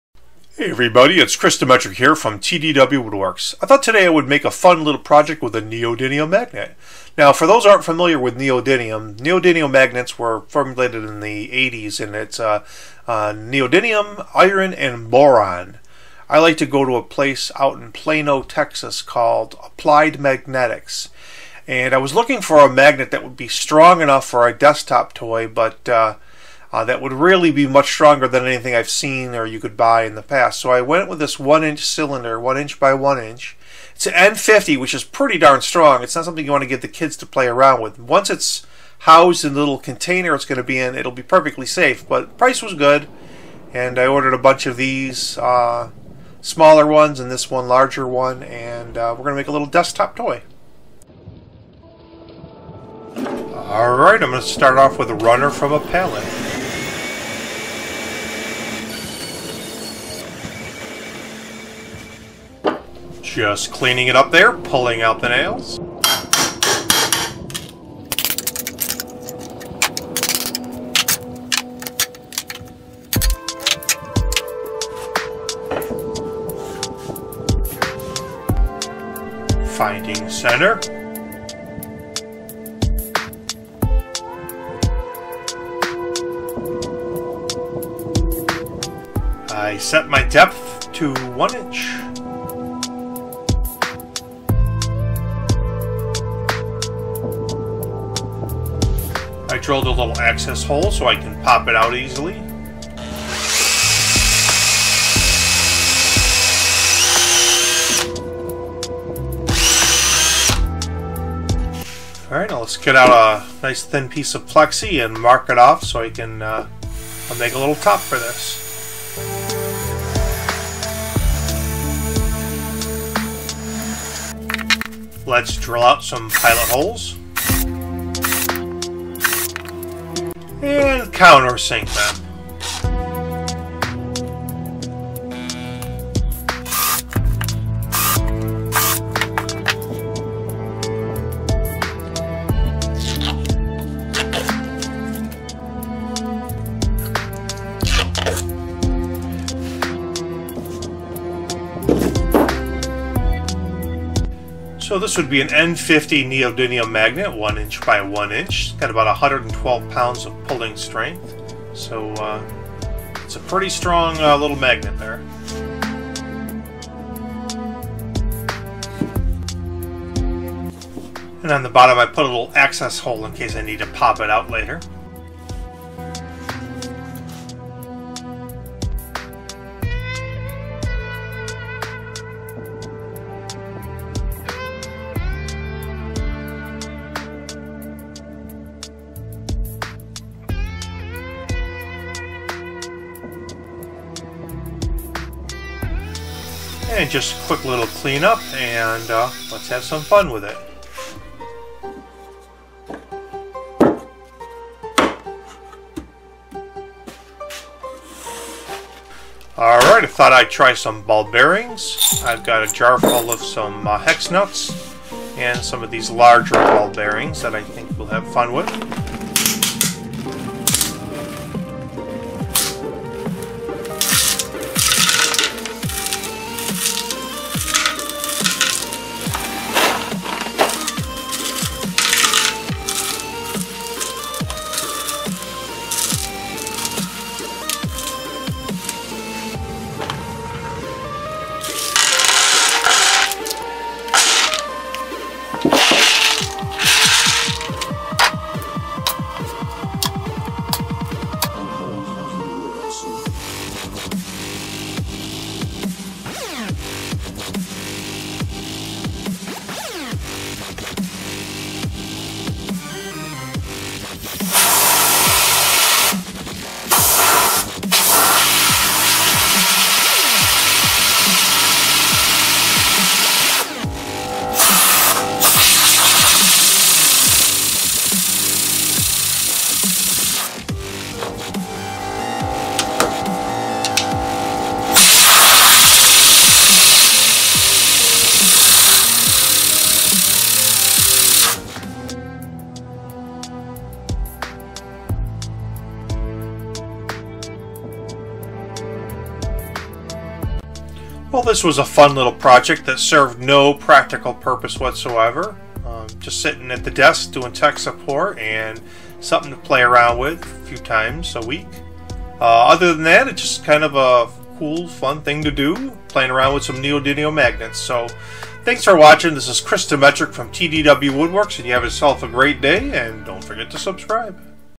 How To Make Neodymium Magnet Desk Toy
Boy and their toys. Just having fun with a neodymium magnet toy.